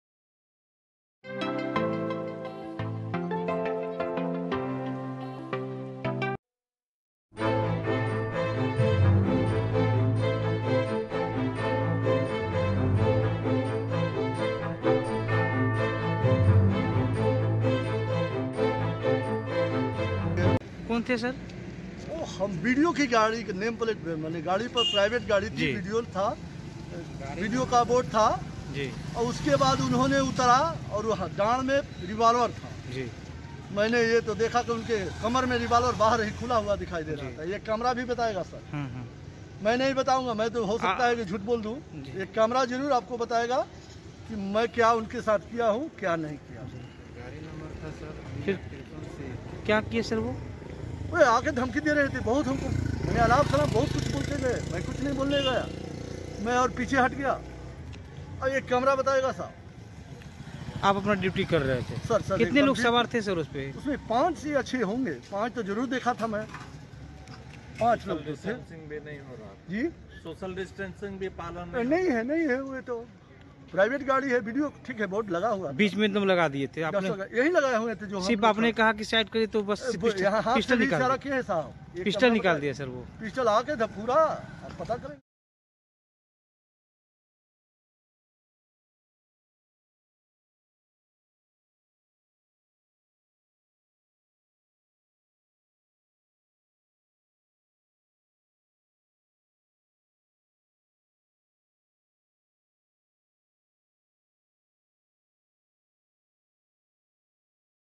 Jamui में Lockdown गाड़ी रोकने पर BDO ने दिखाई दारोगा को पिस्टल
जमुई में ड्यूटी पर तैनात एक दारोगा के सोशल डिस्टेंसिंग के बारे में समझाने पर बीडीयो ने दिखाई पिस्टल। यही नहीं चकाई बीडीओ सुनील चांद ने दारोगा विंध्याचल सिंह को औकात में रहने की नसीहत भी दे डाली। वहीं बीडीओ सुनील चांद ने बताया कि ड्यूटी पर तैनात एसआई जानबूझकर मुझे करते हैं टारगेट।